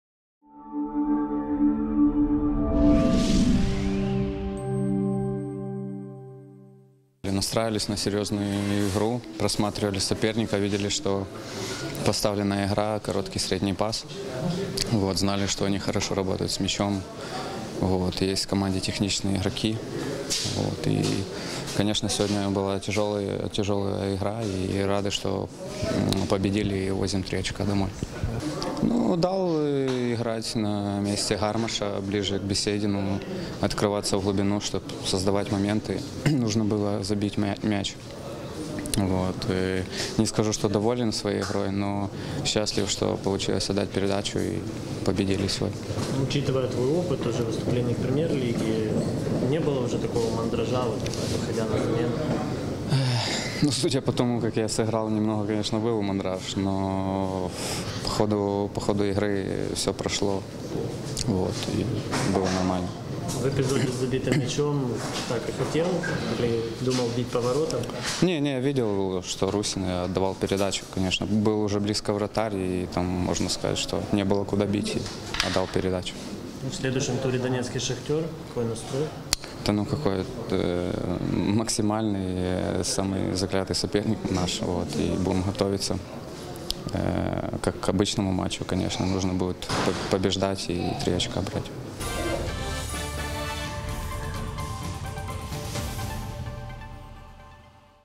Олександр АНДРІЄВСЬКИЙ про матч з ФК "Львів"
Автор гольової передачі прокоментував матч 2-го туру чемпіонату України.